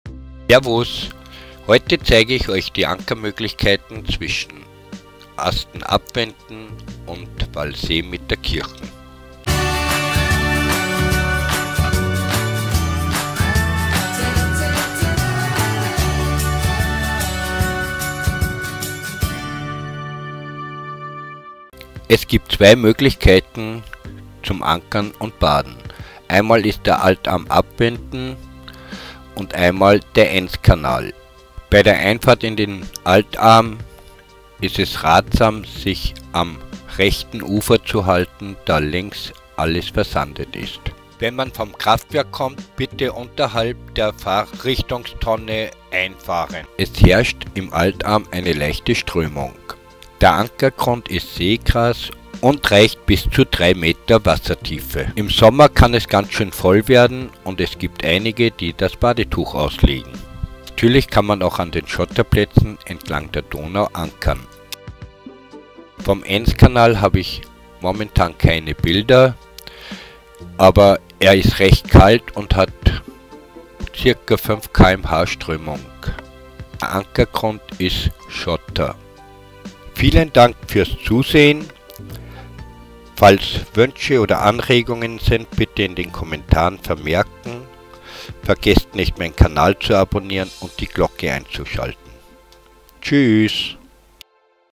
#5 Ankerplätze Abwinden
Mobo Floh
Donaukilometer 2120 bis 22097
Ankerplätze auf der Donau zwischen Abwinden und Mitterkirchen